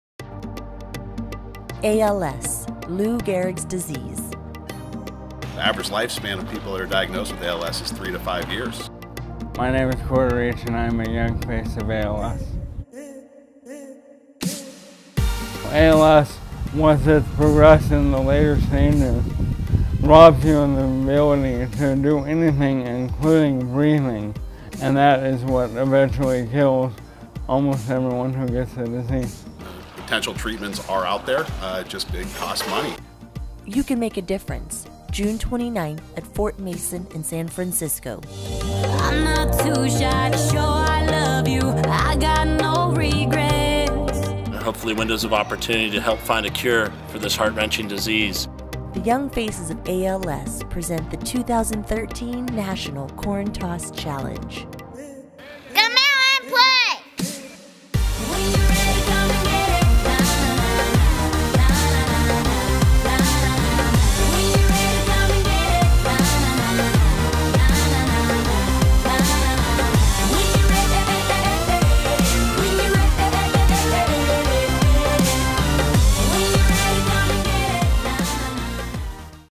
Young Faces of ALS 2013 Corntoss Tourney
What are you doing on Saturday June 29th?   Seriously, come out and play!  Show off your skills, have a great time, and make a difference in the fight against ALS.
YFALS 2013 Corntoss Tourney Fort Mason, San Francisco.  Saturday, June 29th.